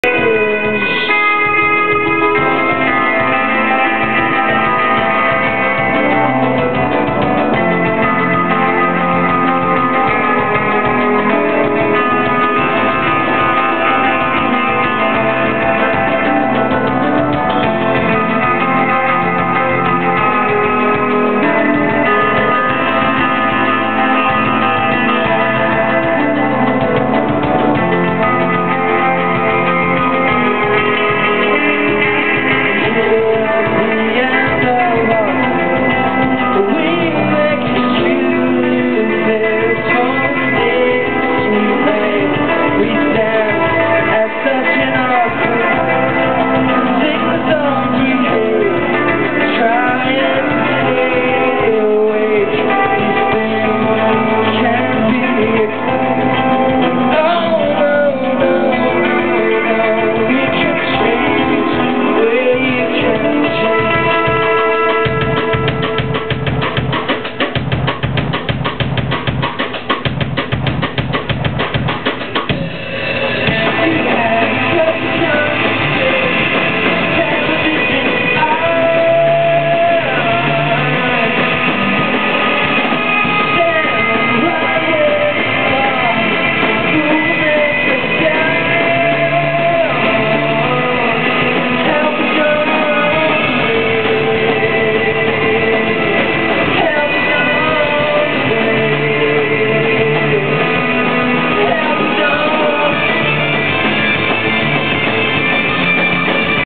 Tourist the band in Seattle at Crocodile Cafe
Sorry for the poor video quality. These guys are amazing live, and it just doent do em justice.